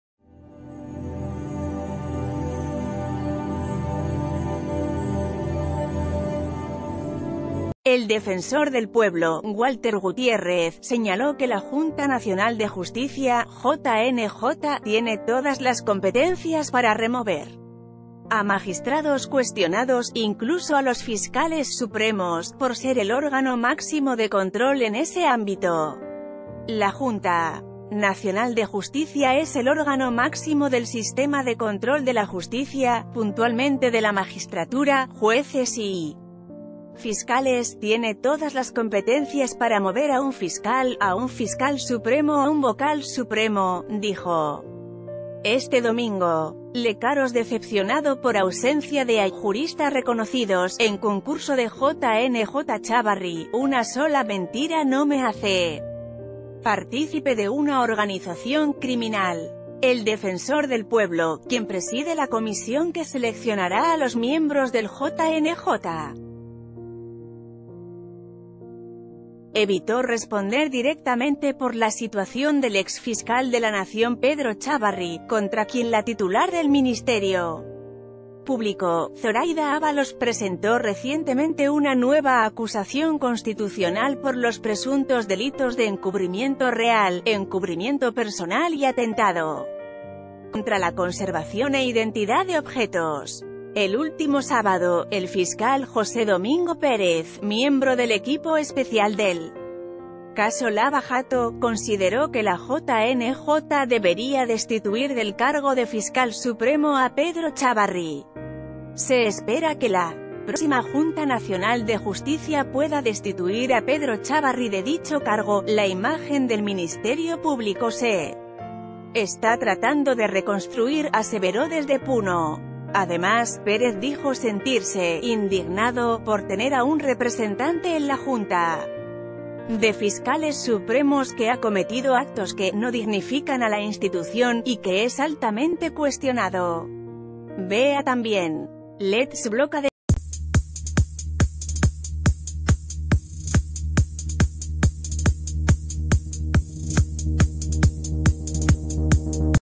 Gutiérrez: La JNJ tiene todas las competencias para mover a un fiscal supremo
El defensor del Pueblo, Walter Gutiérrez, señaló que la Junta Nacional de Justicia (JNJ) “tiene todas las competencias” para remover a magistrados cuestionados, incluso a los fiscales supremos, por se...